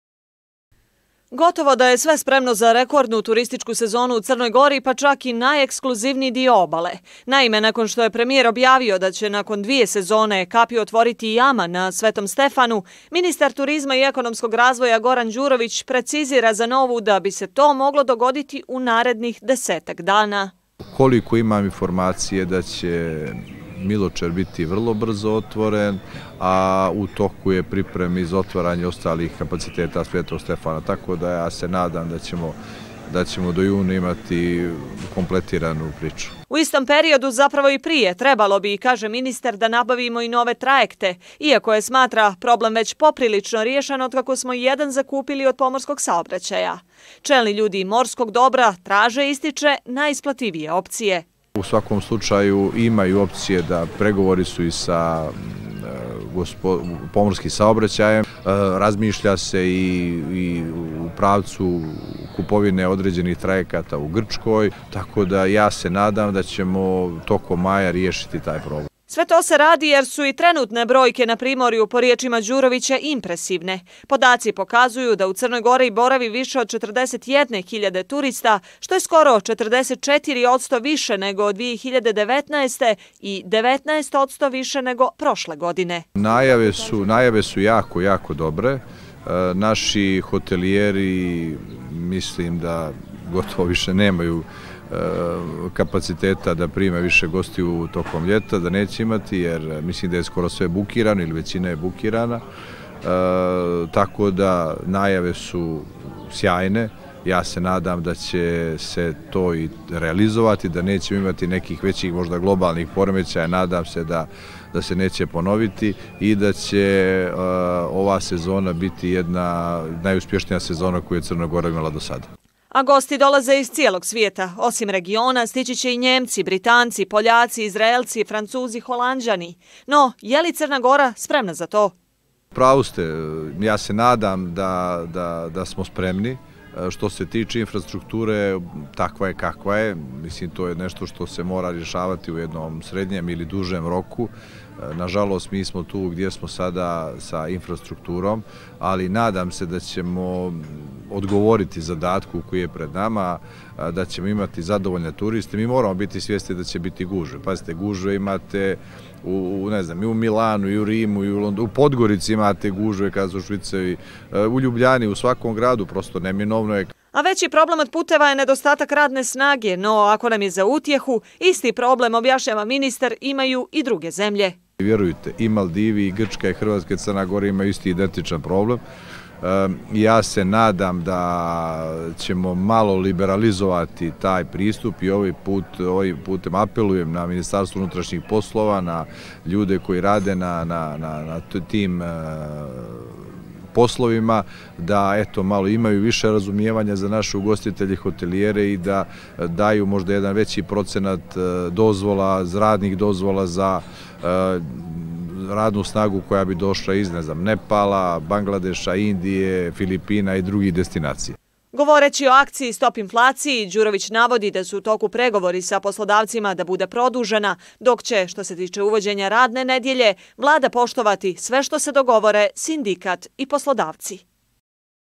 NOVA - VELIKA OČEKIVANJA, MILICA DELIBAŠIĆ MERDOVIĆ 23.05.2023.
Ministar ekonomskog razvoja i turizma Goran Đurović kazao je za TV Nova da bi nove trajekte trebalo da imamo do kraja ovog mjeseca. Do juna će biti otvorena i Vila Miločer, precizirao je on, i kazao da je u toku priprema i za otvaranje ostalih kapaciteta Svetog Stefana. Trenutno je, ističe, u Crnoj Gori više od 40 hiljada turista, a bum nas tek čeka jer se, kaže Đurović, očekuje dolazak turista iz cijelog svijeta.